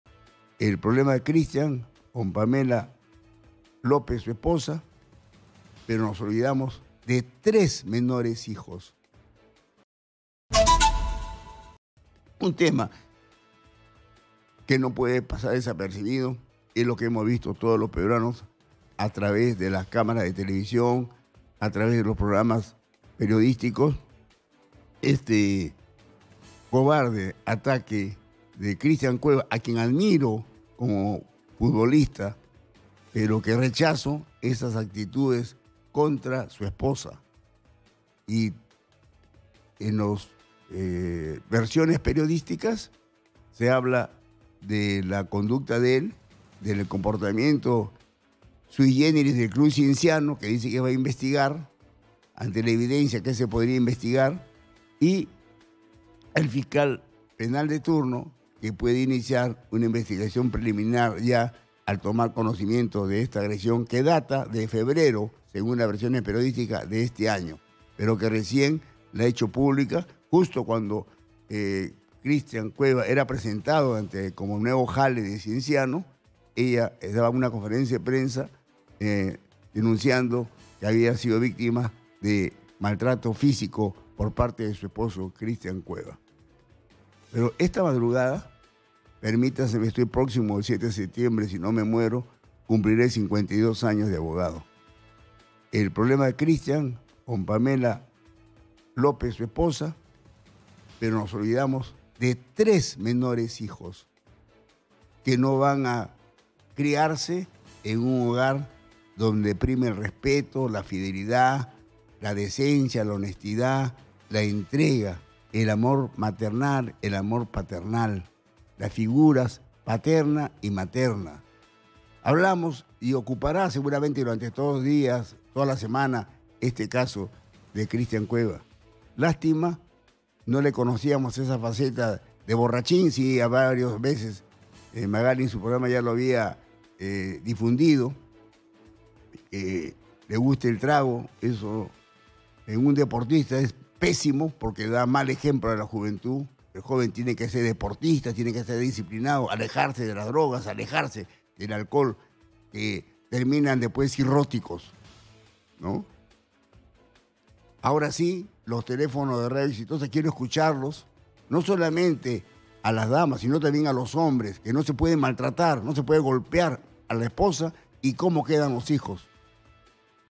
🔴🔵Mario Camacho sobre caso Christian Cueva: "Nos estamos olvidando de los hijos"
El conductor de Exitosa, Mario Camacho, expresó su preocupación por los hijos de Pamela López y Christian Cueva, asegurando que serían afectados tras la denuncia contra el futbolista por agresión a su expareja.

Exitosa Perú
"Exitosa: La voz que integra al Perú"

Corporación Universal